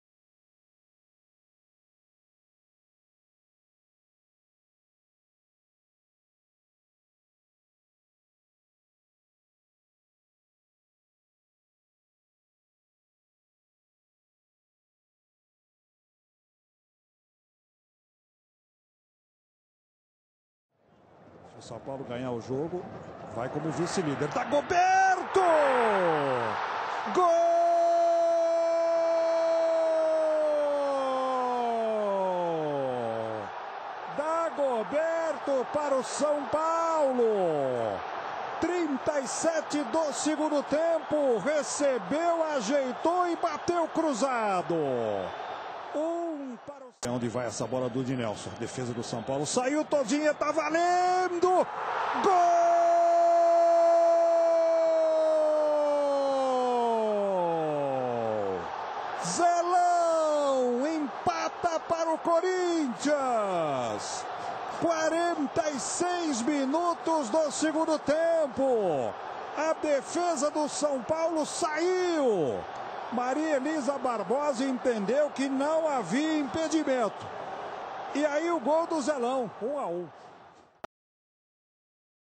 Corinthians 1-1 São Paulo - Campeonato Brasileiro 2007
Jogo da 11ª rodada, no sábado 14/07, disputado no Morumbi, em São Paulo.

Corinthians-SP 1-1 São Paulo-SP
(82' Dagoberto [S]; 90+1' Zelão [C])

Futebesteirol